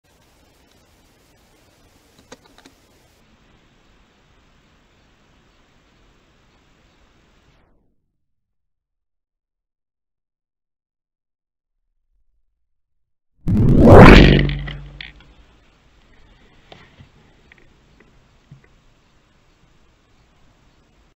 Slow MO crossbow
First DOE I sitting down doe came up hill with another not found blood but in spring found arrow 100yards away with hair in it